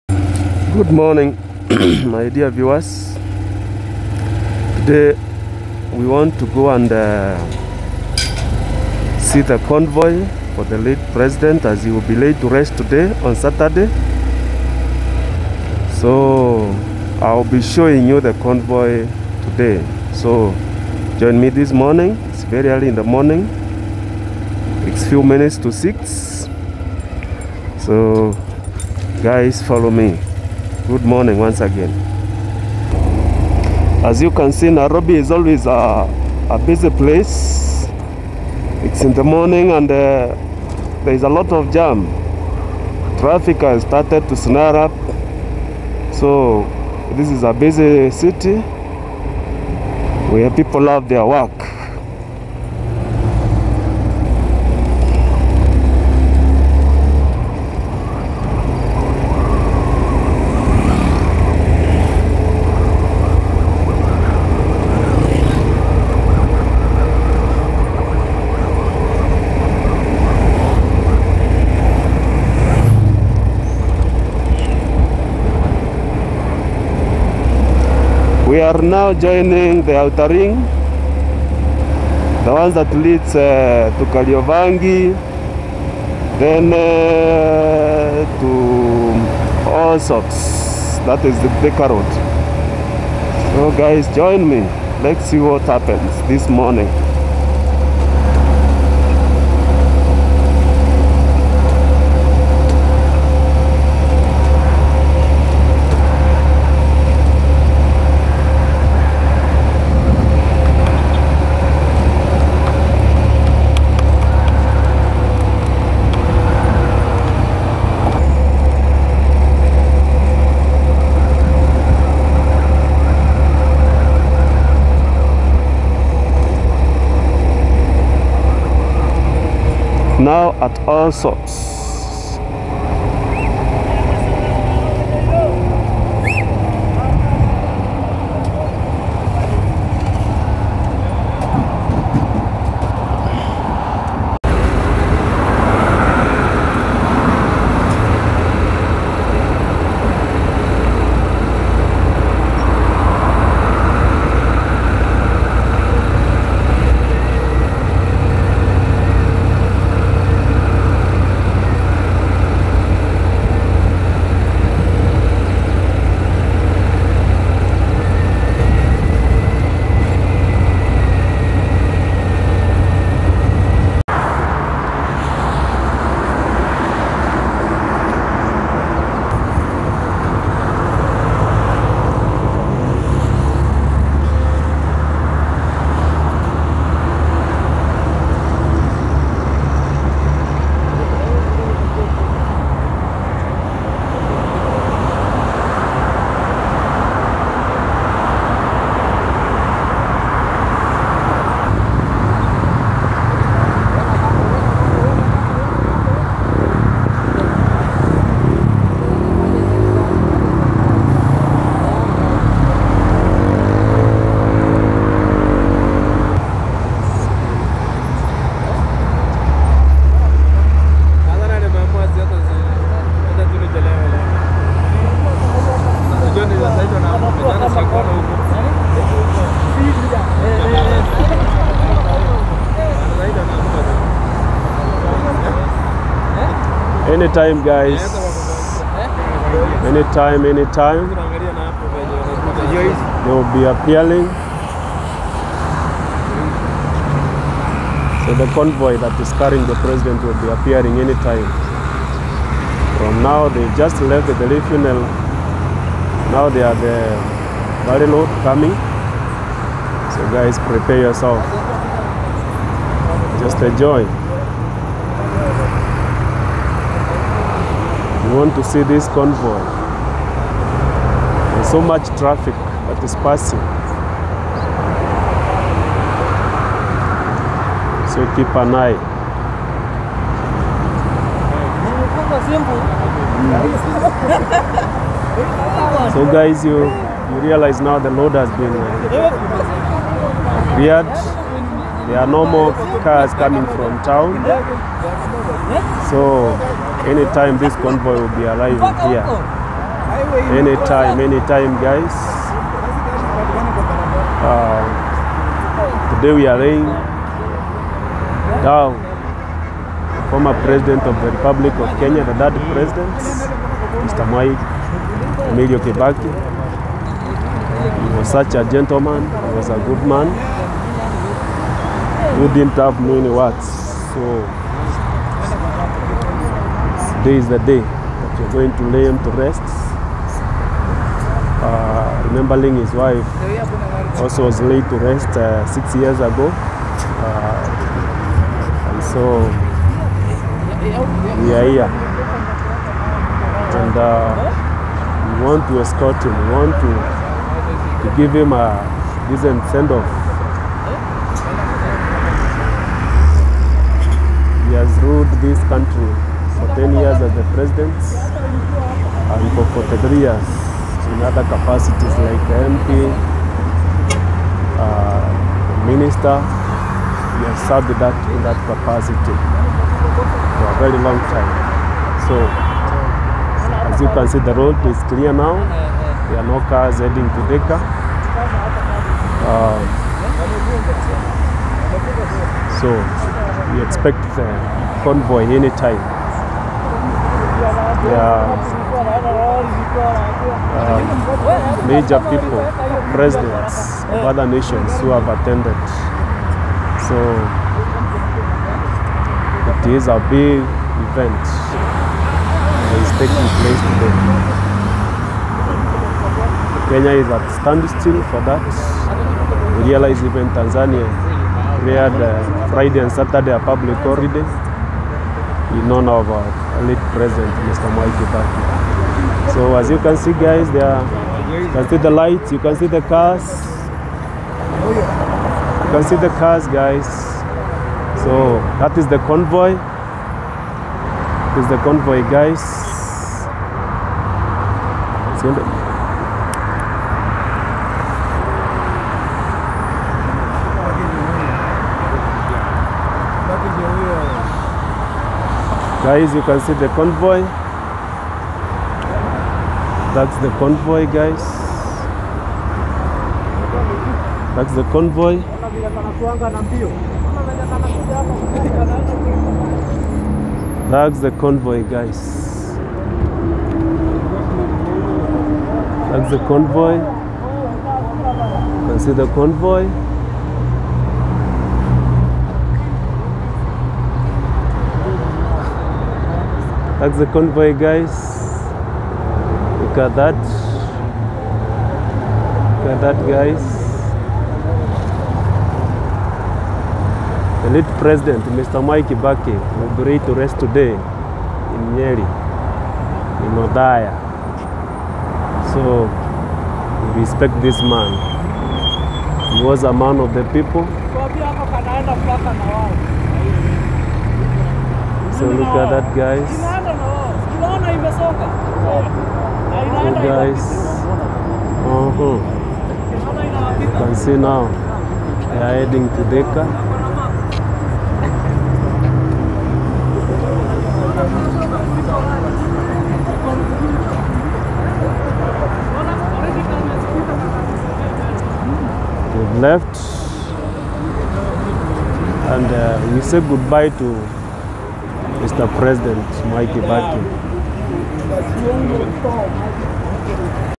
Mwai kibakis final send off convoy at his home in Othaya Nyeri county .
Rest in peace our beloved 3rd president Mr. Mwai Kibaki.